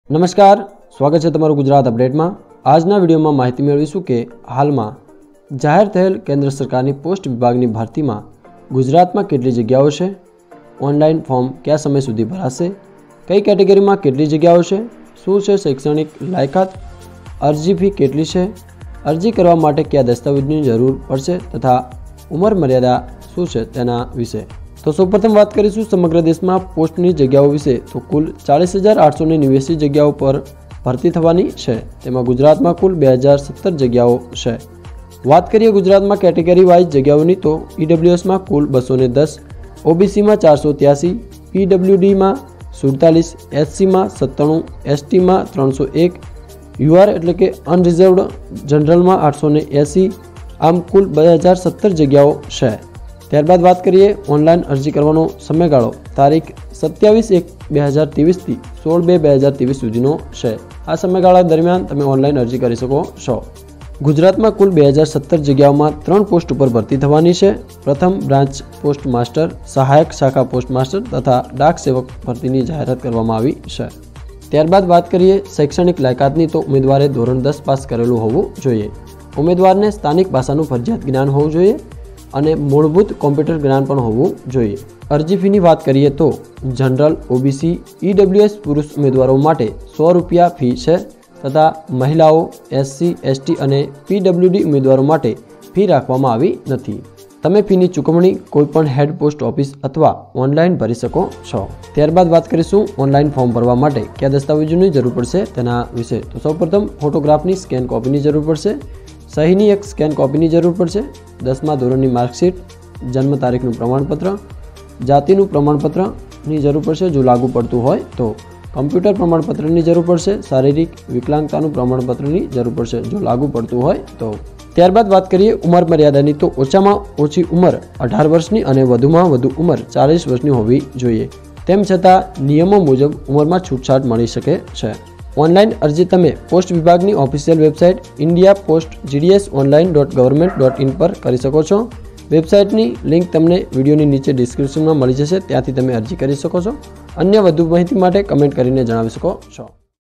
ઈન્ડિયા પોસ્ટ ભરતી 2023 | ગુજરાતમાં કેટલી જગ્યા, લાયકાત, દસ્તાવેજ જાણો સંપુર્ણ માહિતી | India Post
કેન્દ્રસરકાર દ્વારા પોસ્ટની ભરતી કરવામાં આવી છે આ ભરતીમાં ગુજરાતમાં કુલ કેટલી જગ્યાઓ છે, કઈ કેટેગરીમાં કેટલી જગ્યા છે, કેટલી ફી છે, કયાં કયાં દસ્તાવેજોની જરૂર પડશે તેની સંપુર્ણ માહિતી આ વીડિયો માં મળી જશે.